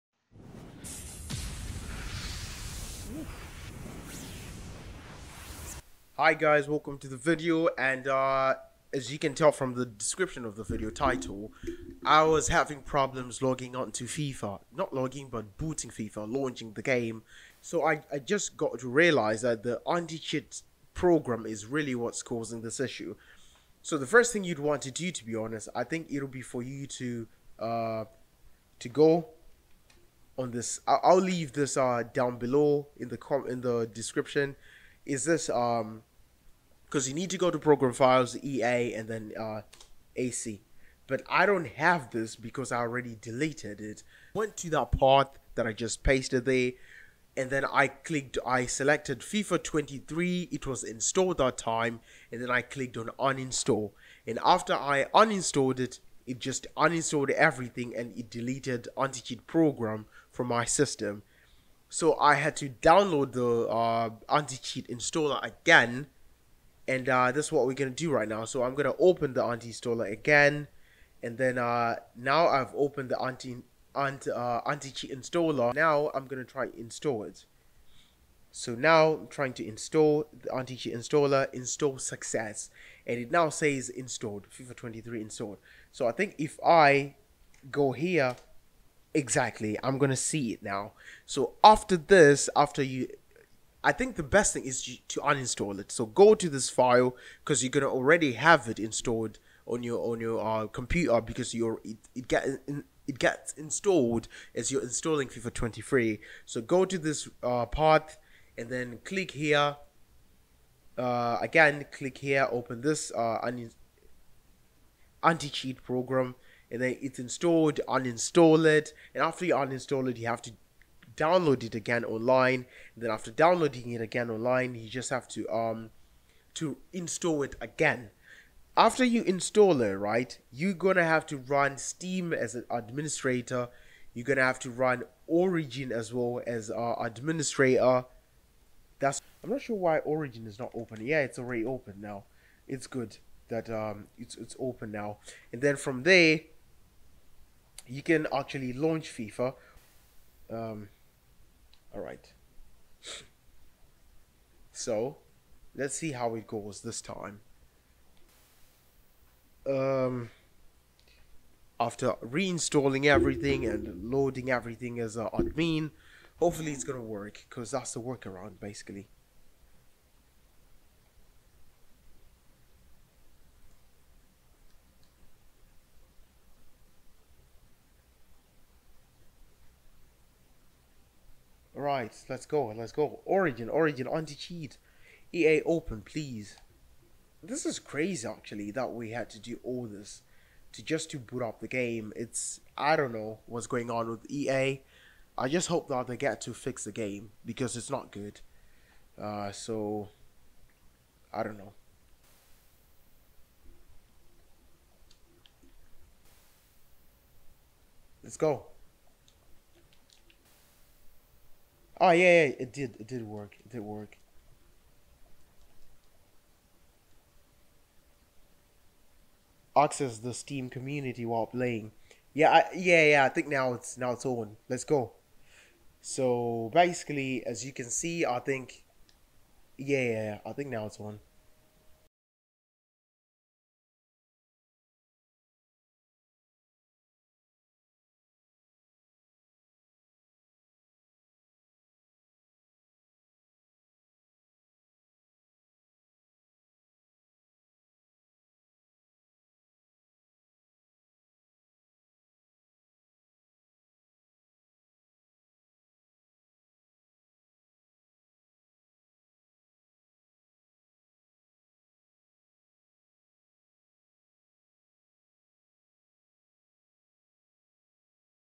FIFA 23 PC Anti Cheat Crash FIX 100% working
Where to find anti cheat app : C:\Program Files\EA\AC\